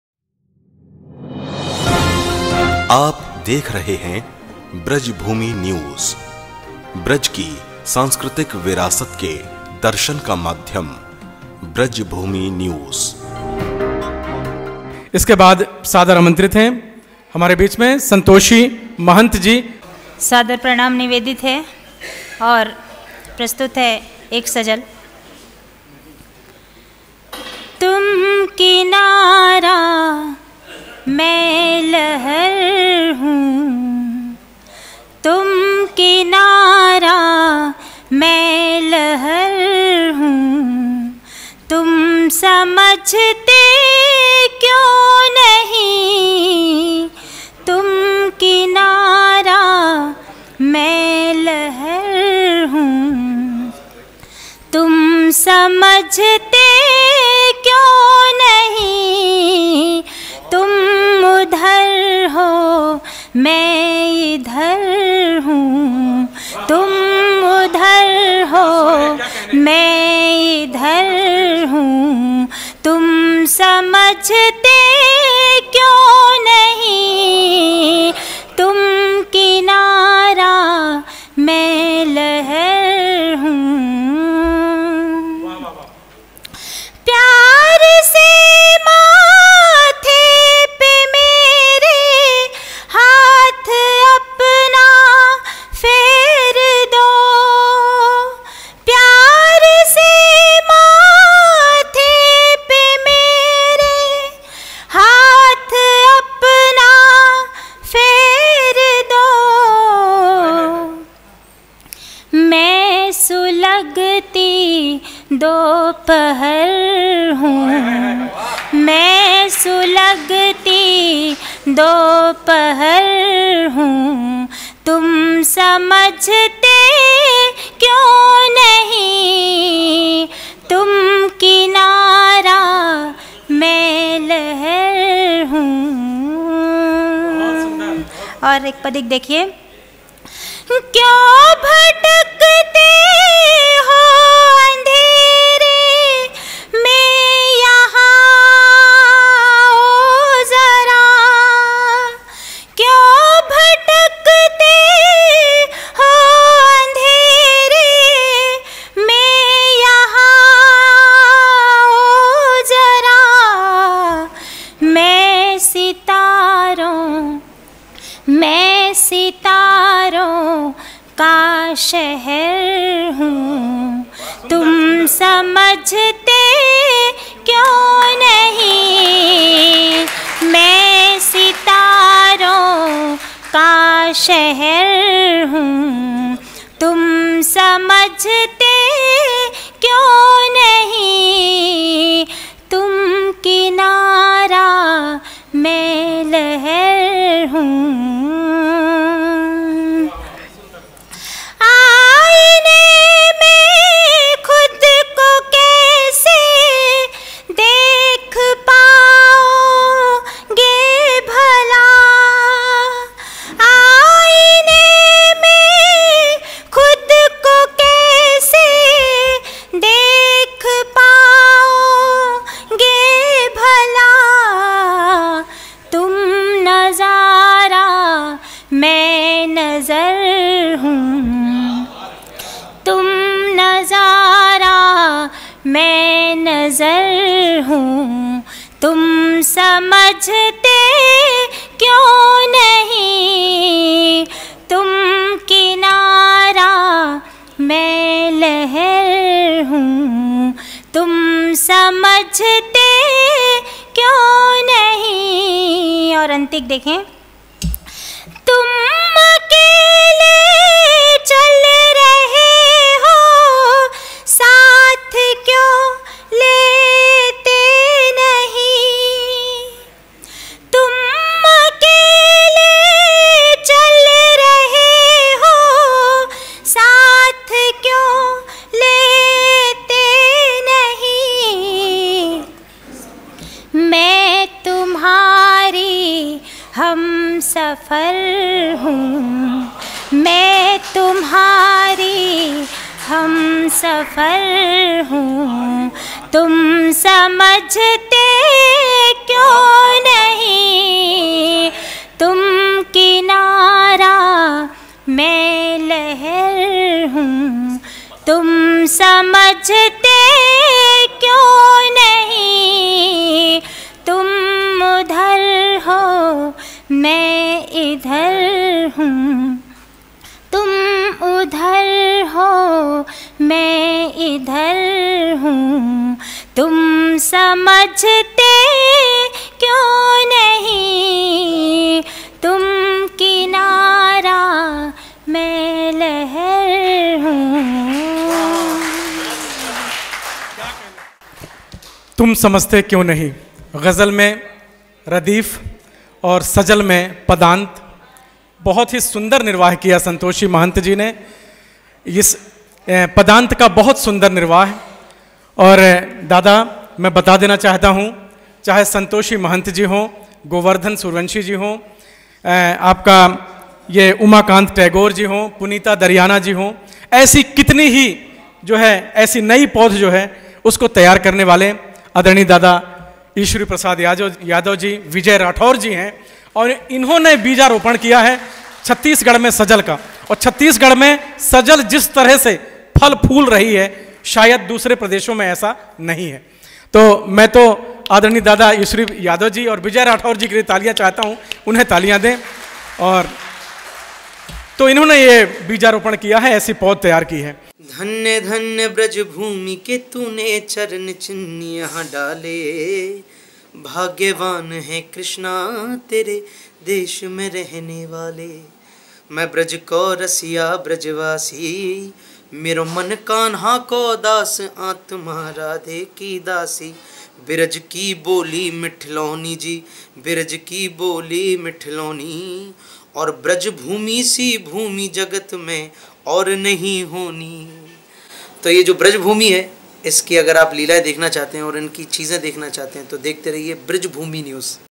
संतोषी महंत "श्रद्धा" - कविता पाठ
चतुर्थ वार्षिक सजल समारोह, मथुरा
१६ अक्तूबर २०२२
आयोजक - हिंदी सजल सर्जना समिति, मथुरा